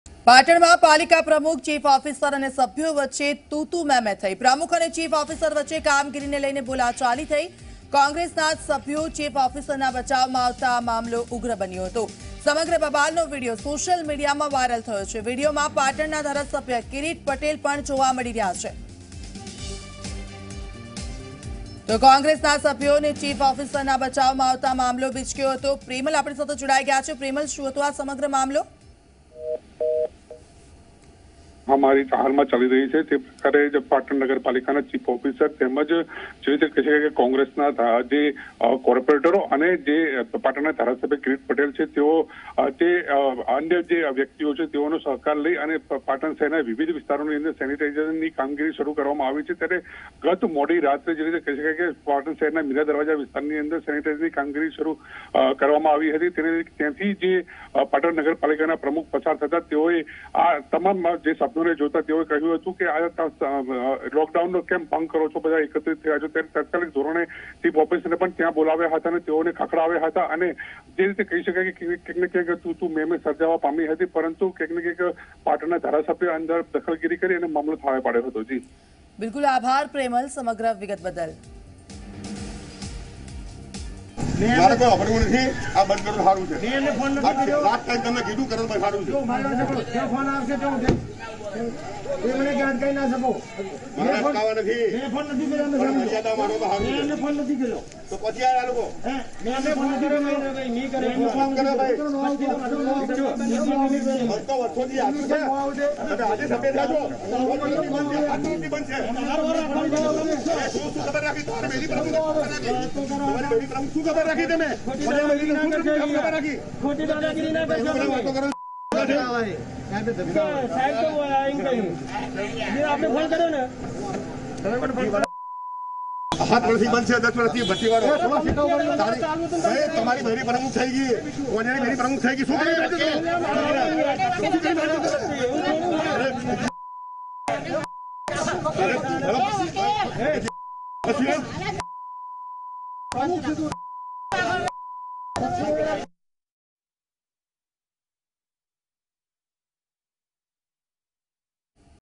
Patan | Gujarat | Fighting | પાટણ | chief officer
પાટણમાં પાલિકા પ્રમુખ, ચીફ ઓફિસર અને સભ્યો વચ્ચે બોલાચાલીનો માહોલ સર્જાયો છે.કોરોના વચ્ચે આ બબાલે ગંભીર સ્વરૂપ લીધું હતું.